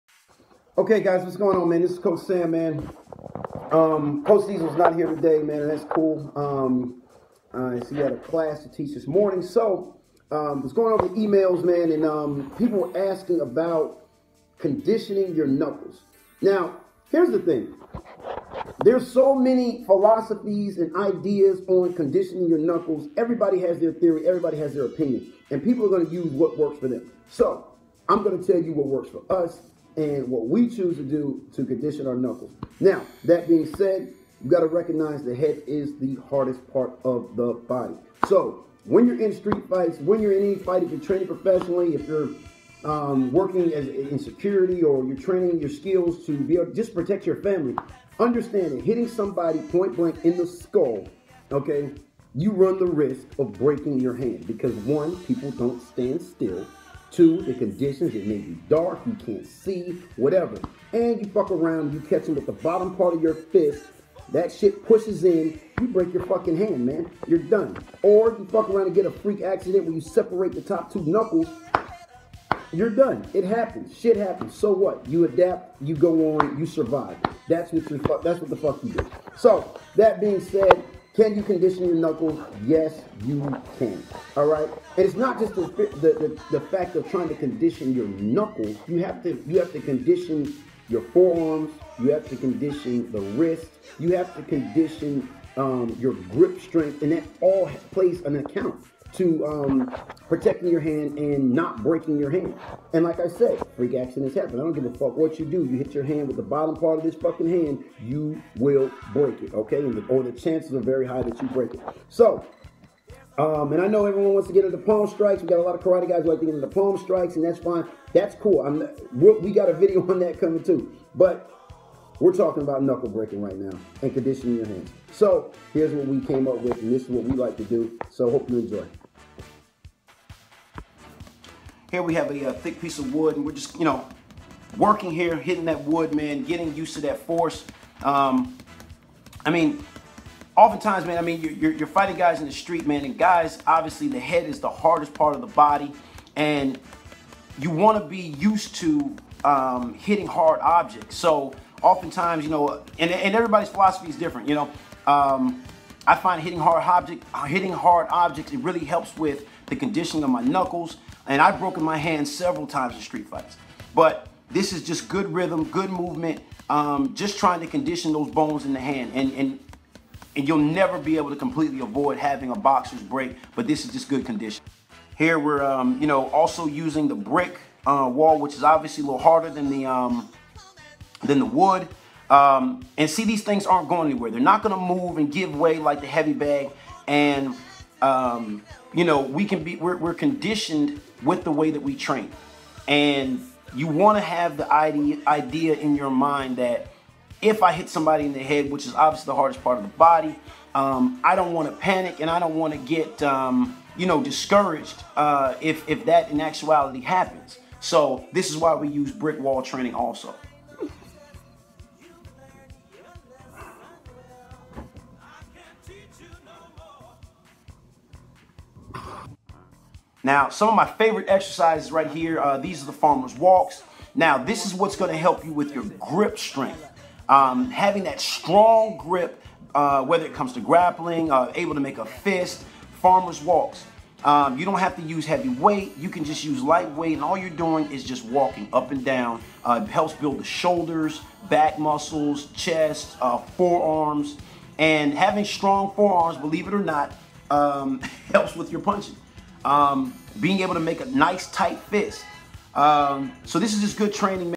HOW TO BUILD YOUR KNUCKLES UP TO WIN STREET FIGHTS
TRAINING YOUR KNUCKLES WRISTS FOREARMS AND GRIP THROUGH BOARD BRICK KNUCKLE PUSH UPS AND FARMERS WALK TRAINING